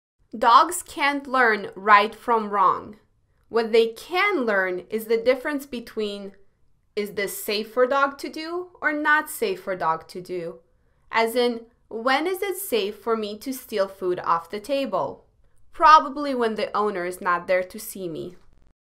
Dogs Don't Learn Right vs Wrong
Dogs can’t learn right from wrong. What they can learn is the difference between is this safe for dog to do or not safe for dog to do. As in, when is it safe for me to steal food off the table? Probably when the owner is not there to see me!

For more videos like this one, along with many more resources for learning to communicate with your dog, please visit: